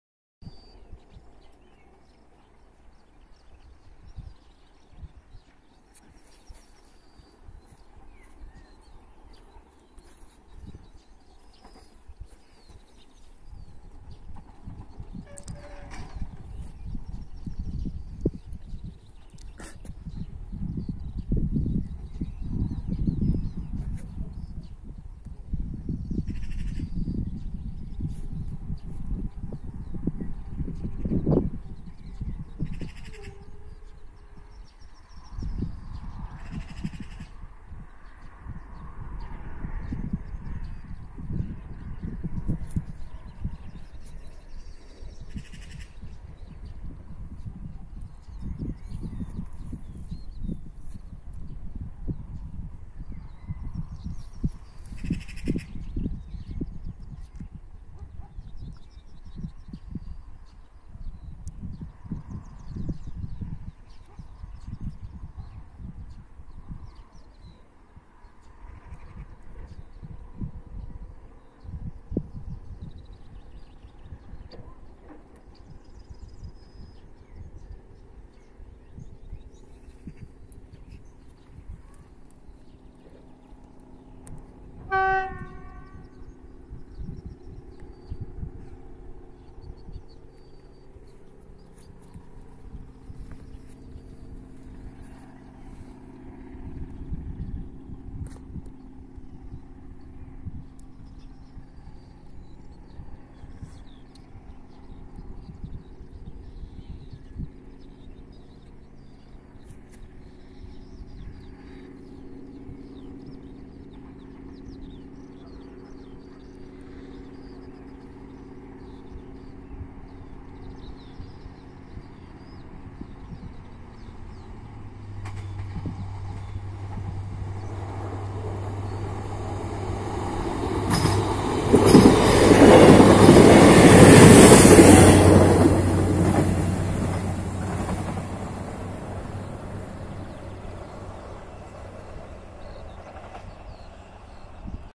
SA123 - 004 + SA106 - 016 ARRIVA | Serock
SA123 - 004 i SA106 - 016 ARRIVA jako AR50283

RELACJA: Bydgoszcz Główna - Tuchola

Co ciekawe , stacja kolejowa w Serocku jest jedyną czynną stacją na odcinku lk. 201 ( Nowa Wieś Wielka - Gdynia Port ) - Maksymilianowo - Wierzchucin . Dodatkowo posiada ona semafory kształtowe ( Już obecnie są one ,,gatunkiem" ginącym ) oraz urządzenia mechaniczne scentralizowane . Jedynie semafor wjazdowy i tarcza ostrzegawcza od strony Maksymilianowa są świetlne . Kiedyś na odcinku od Maksymilianowo - Wierzchucin stacje były jeszcze w Stronnie ,  Świekatowie oraz Błądzimiu ( Wszystkie te stacje były wyposażone w semafory kształtowe ) ale niestety one już są dawno polikwidowane i zamienione na przystanki osobowe :(